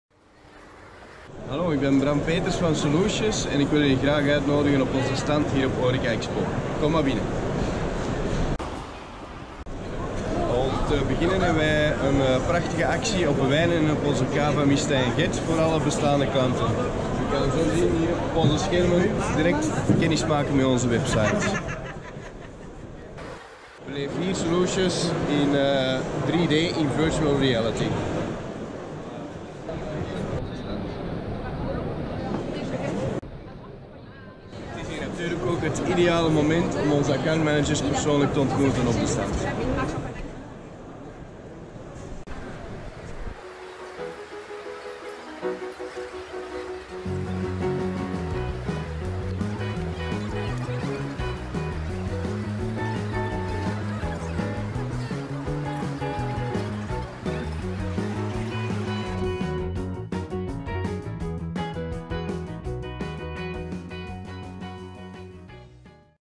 Solucious op Horeca Expo 2019
Benieuwd wat je allemaal kan beleven op onze stand op HorecaExpo? 🍴 Onze collega Bram geeft je met plezier een rondleiding.